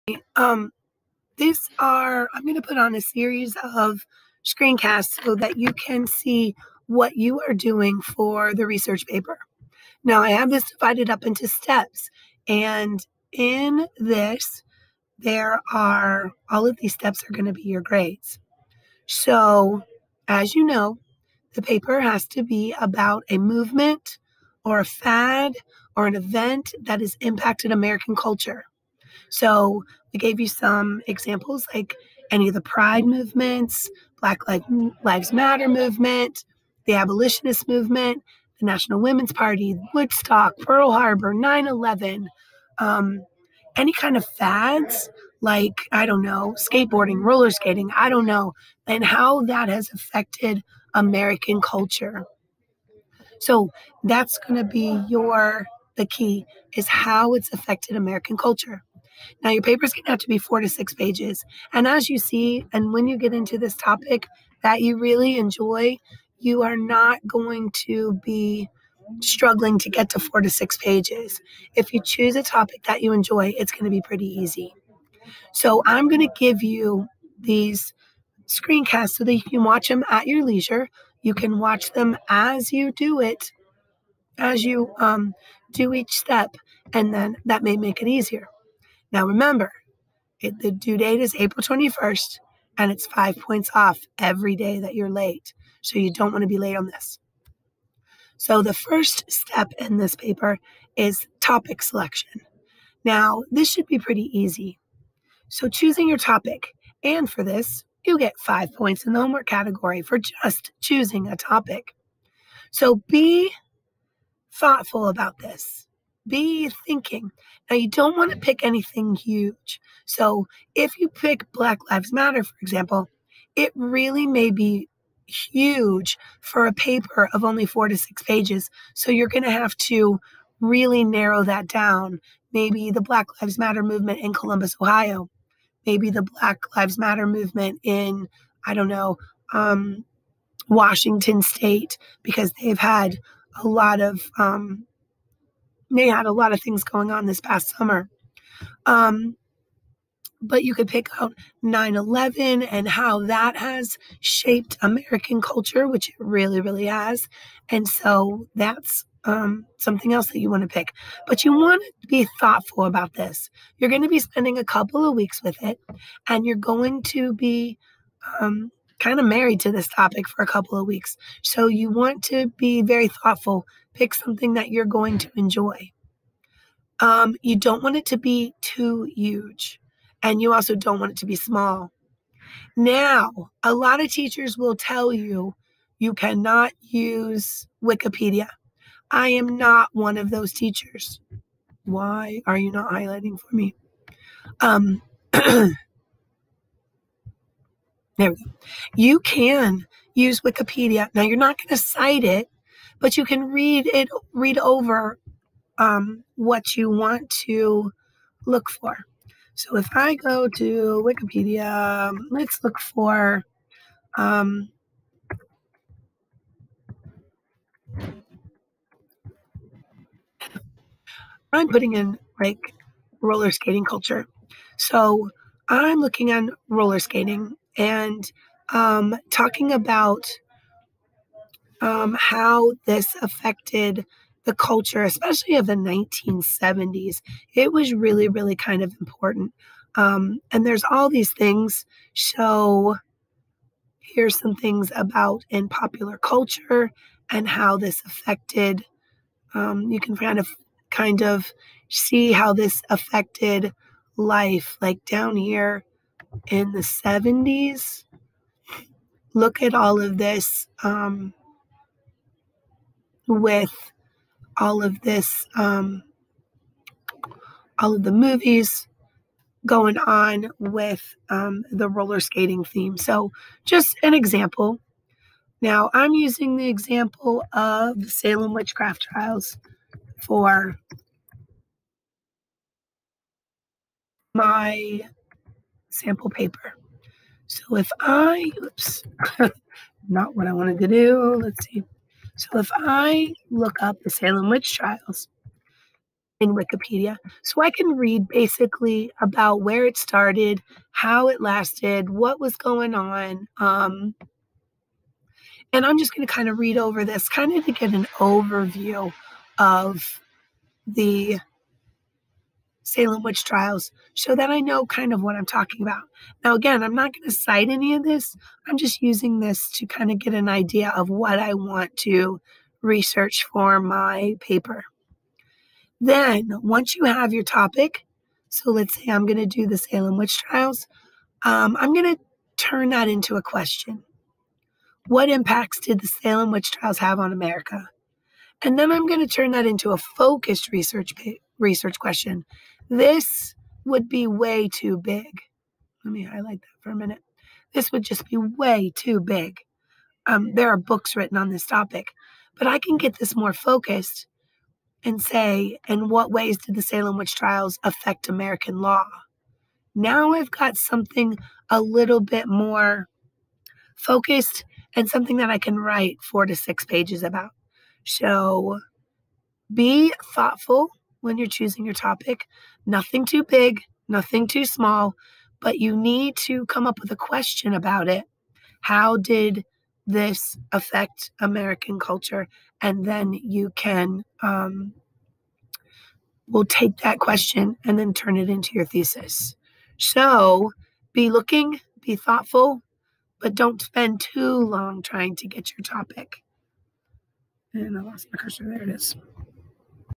Step one - Choosing a topic
Here is a quick video explaining how to find a topic.